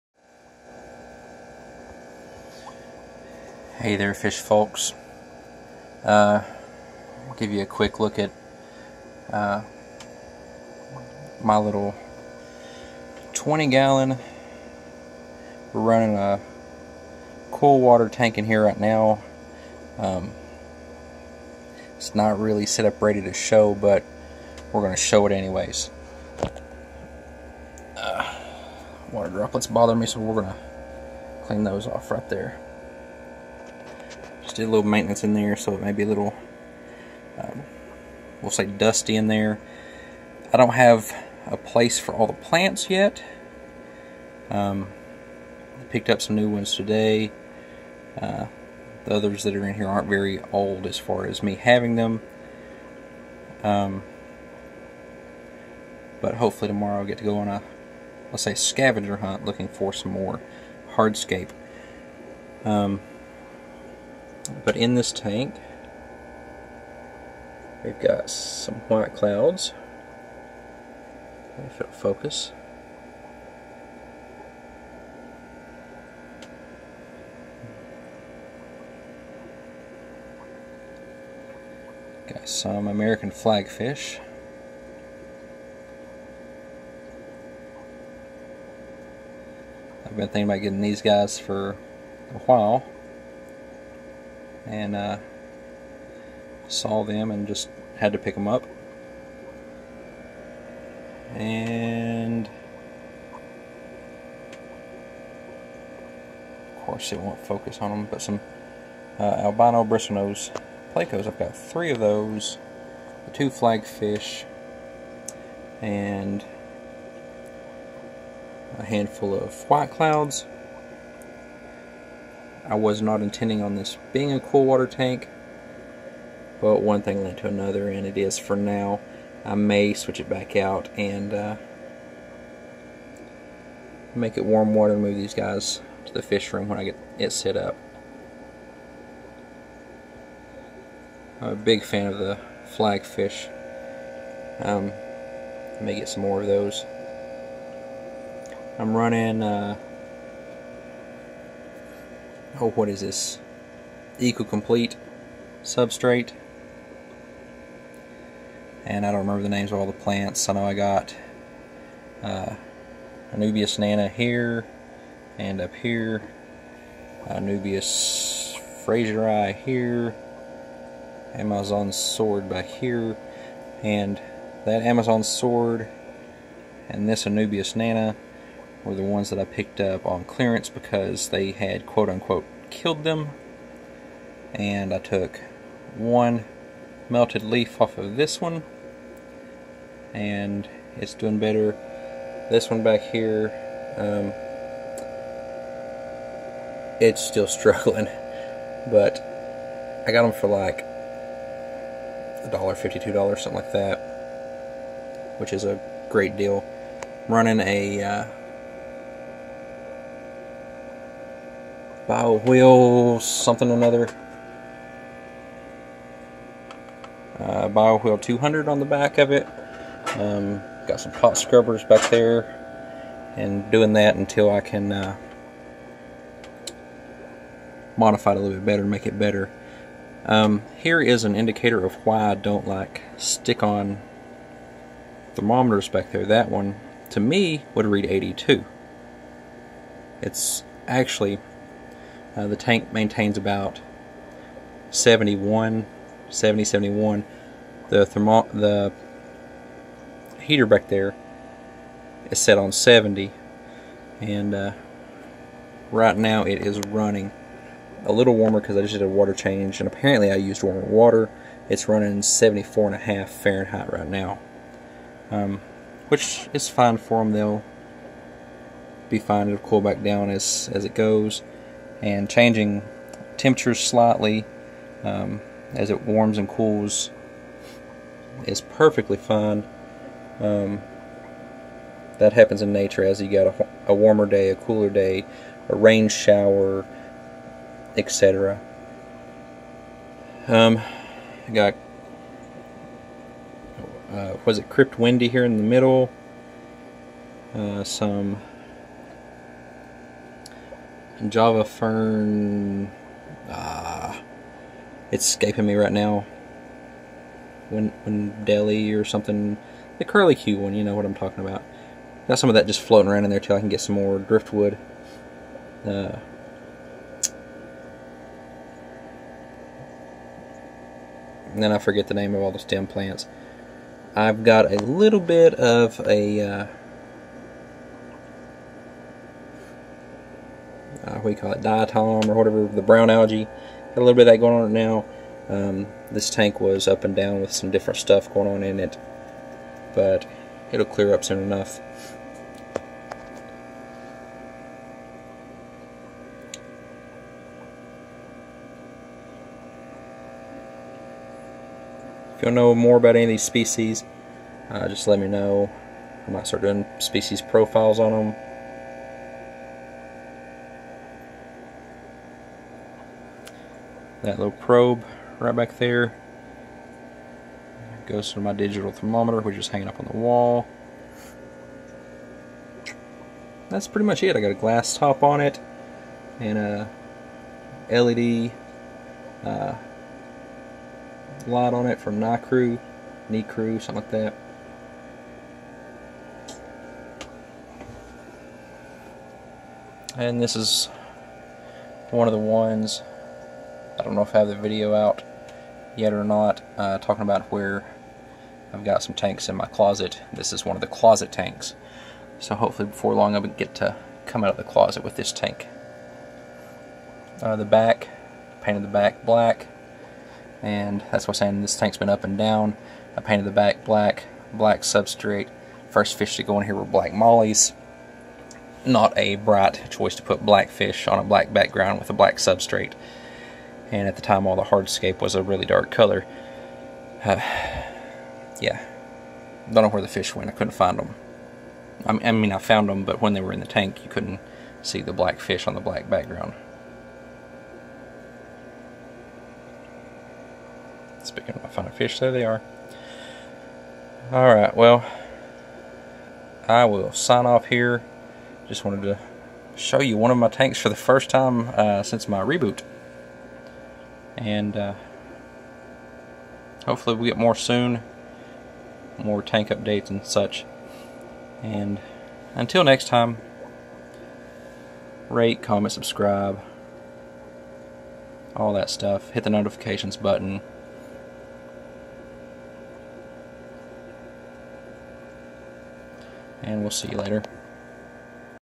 My cool water tank
A quick peek at my 20 long planted cool water tank.